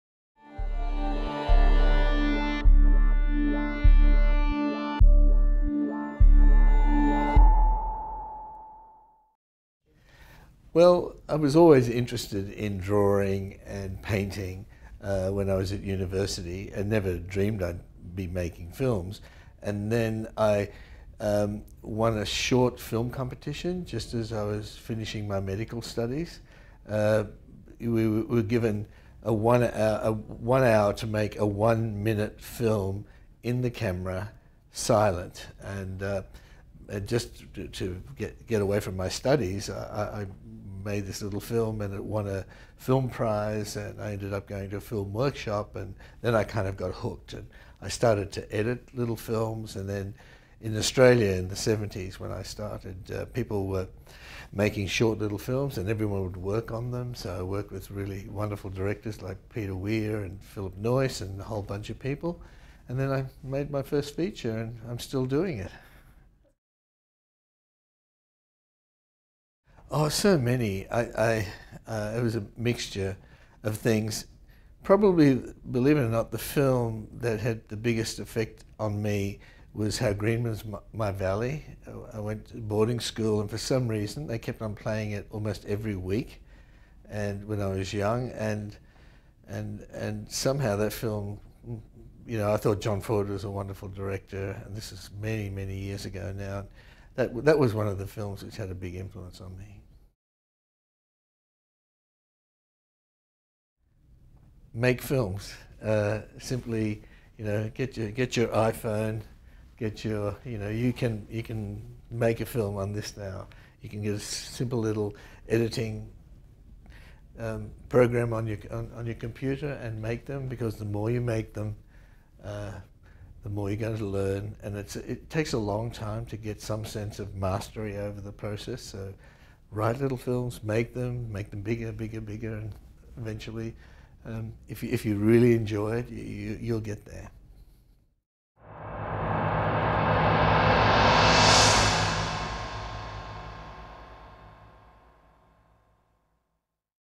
George Miller - "The More You Make Films, The More You Learn"
The director of Mad Max: Fury Road, Happy Feet & Babe answers BAFTA's three Big Questions.

- How did you get started in the industry?
- Which film/TV show/video game has most influenced you?
- What advice would you give to someone starting out?